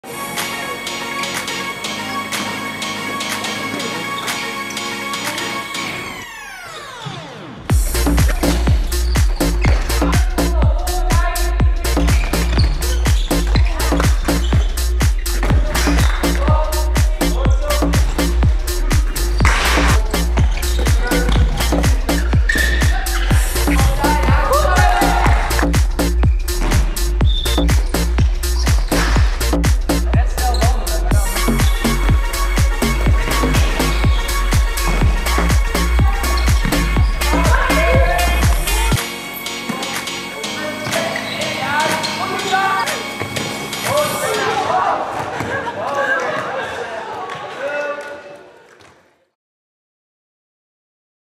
Training op de HandbalAcademie
Op de HandbalAcademie op Papendal trainen talentvolle handballers dag in dag uit om alles uit zichzelf en hun carrière te halen.